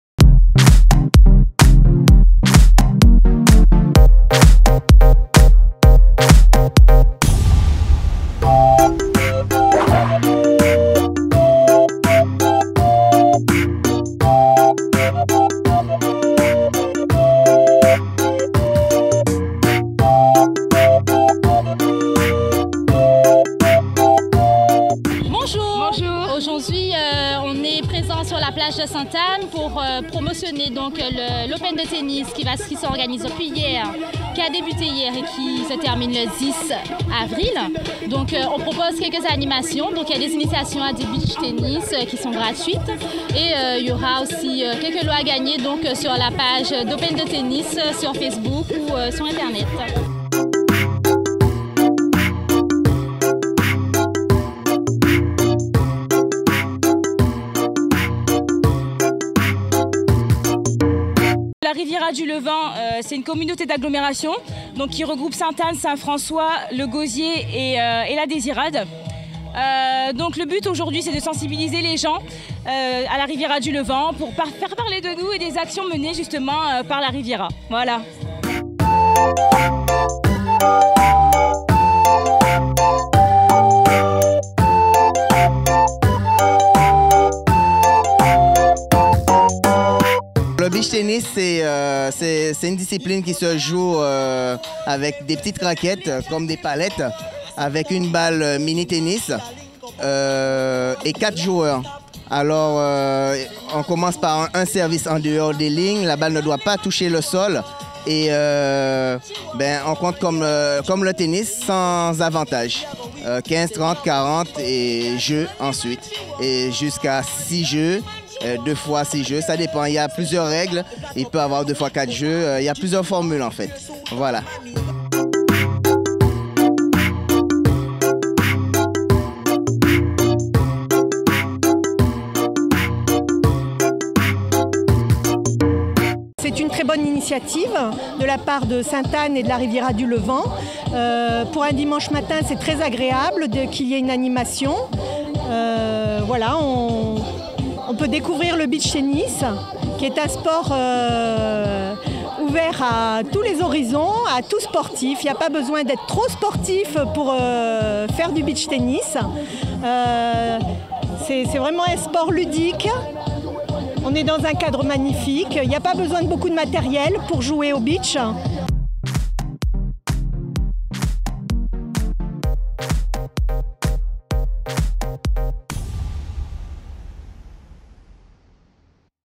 Initiatiation au beach tennis
Revivez l'open de tennis avec la Riviera du Levant sur la plage de Sainte-Anne.